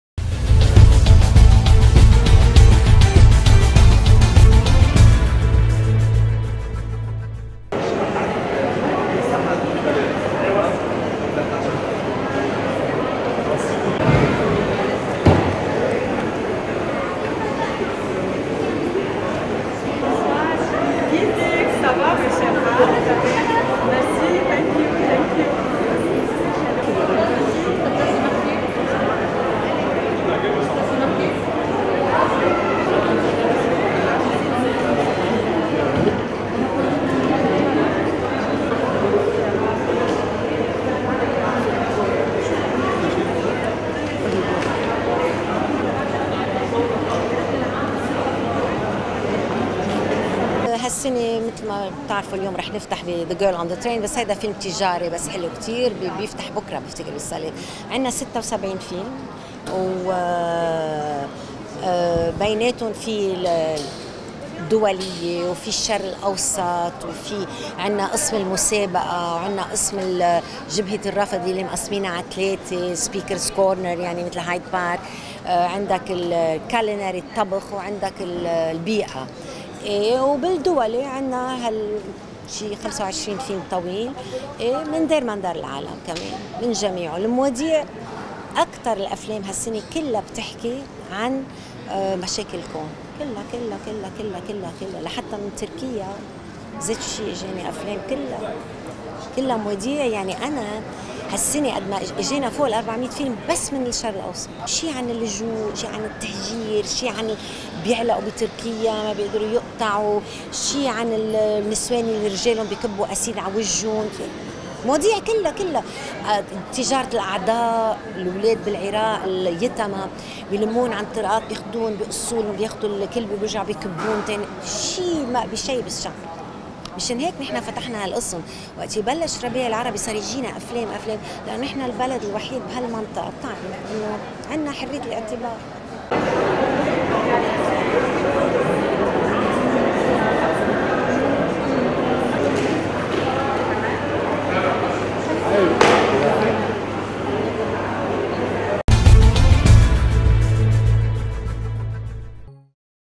مصر العربية | مهرجان "بيروت الدولي للسينما" ينطلق بمشاركة 76 فيلماً
انطلقت في العاصمة اللبنانية بيروت، اليوم الأربعاء، الدورة الـ16 لمهرجان "بيروت الدولي للسينما"، بمشاركة 76 فيلماً، بينها 31 فيلماً قصيراً، وأفلام لمخرجين عالميين حصدوا العديد من الجوائز في مهرجانات دولية.